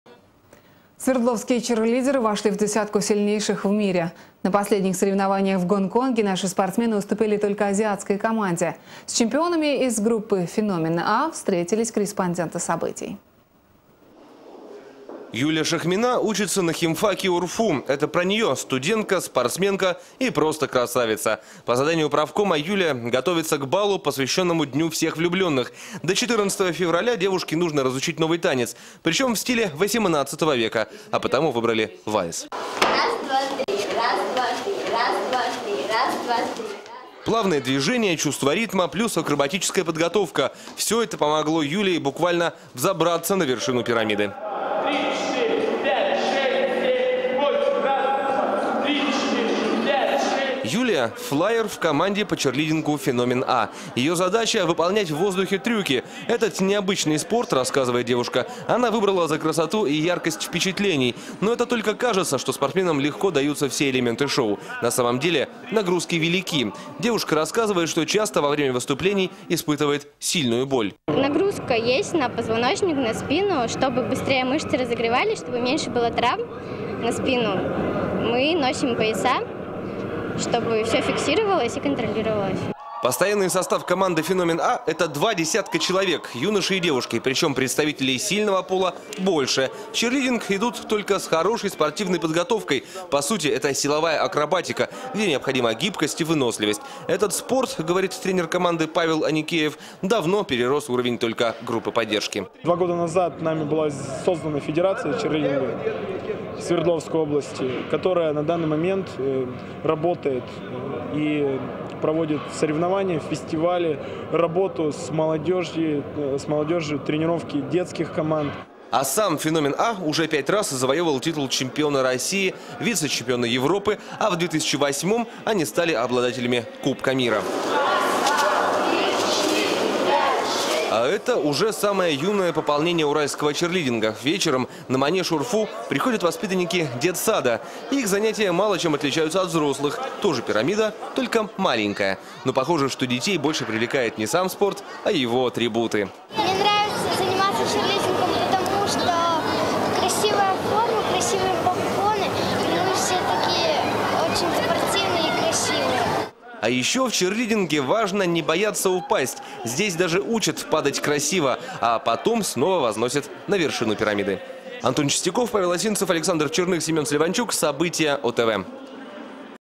Свердловские черлидеры — в десятке сильнейших
На последних соревнованиях в Гонконге наши спортсмены уступили только азиатской команде. С чемпионами из группы «Феномен-А» встретились корреспонденты «Событий».